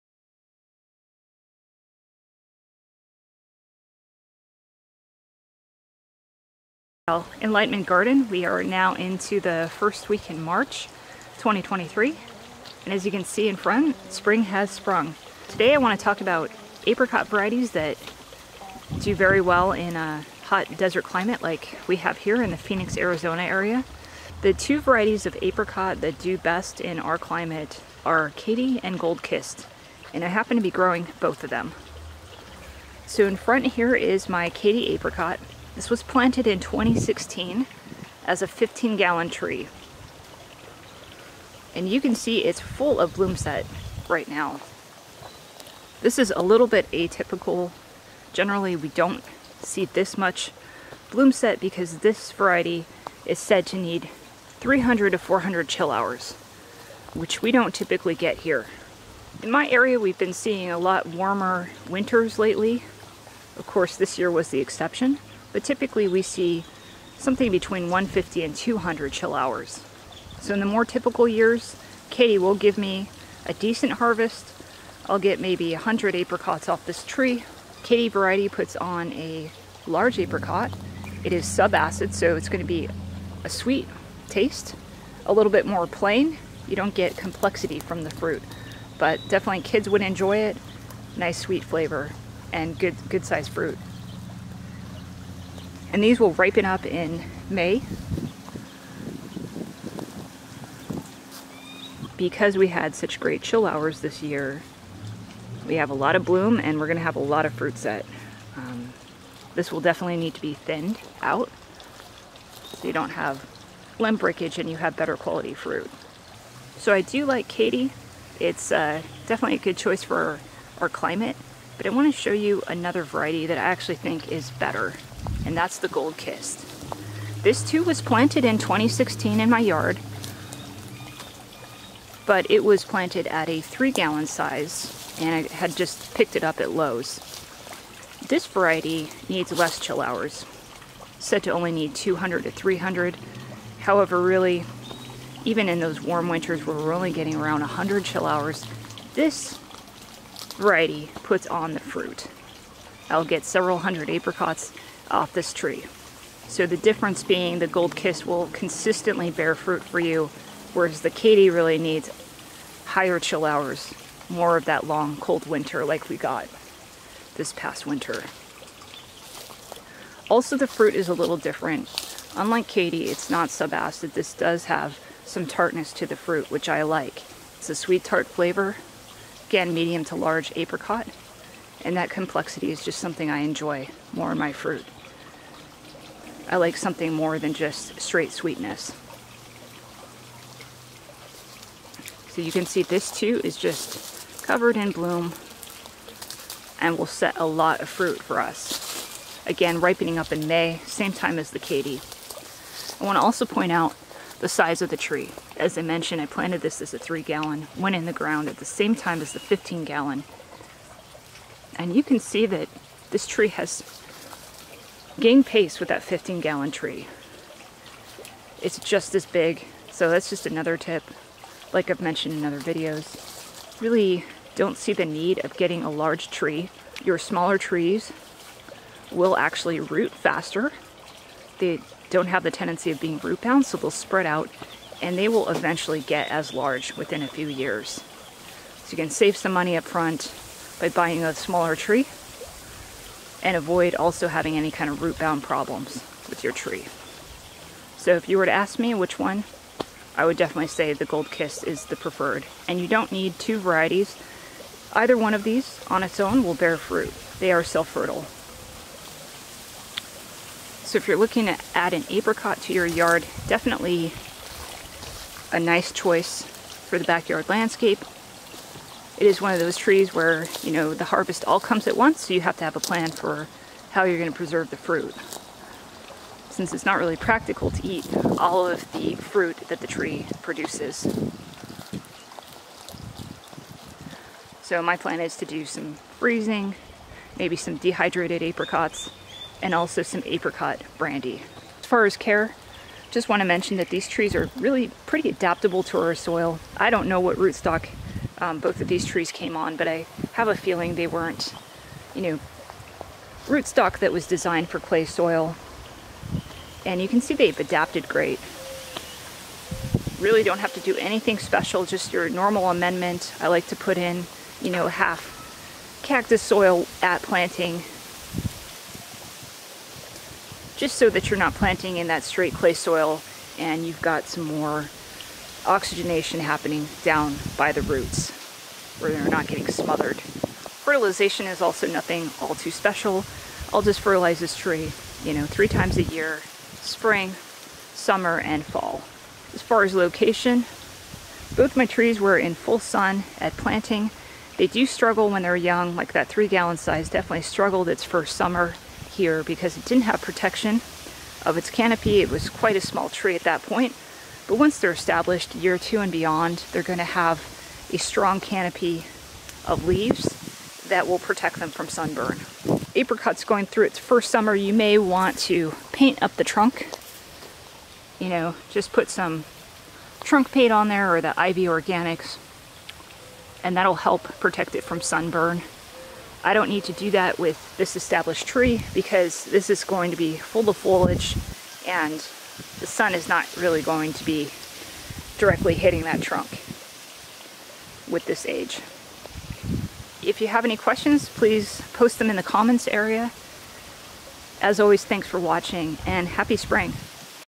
Apricot Variety for a Hot Desert Climate/Arizona 9B
Apricots grow very well in a hot desert climate but the variety makes all the difference. Here I show you 2 mature apricots that have been in ground 7 years in my Phoenix Arizona area yard: Katy and Gold Kist

Watch to learn more about each and which is my favorite!